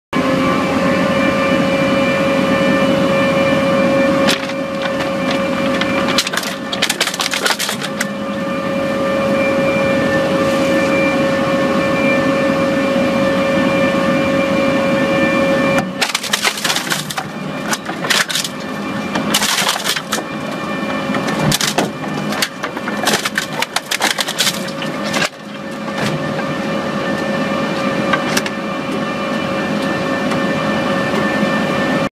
2001 HAMMEL VB750DK For Sale
Title: 2001 HAMMEL VB750DK For Sale

Description: Rebuilt 2001 Hammel Shredder. Financing available.Category: Scrap Processing / Demolition Equipment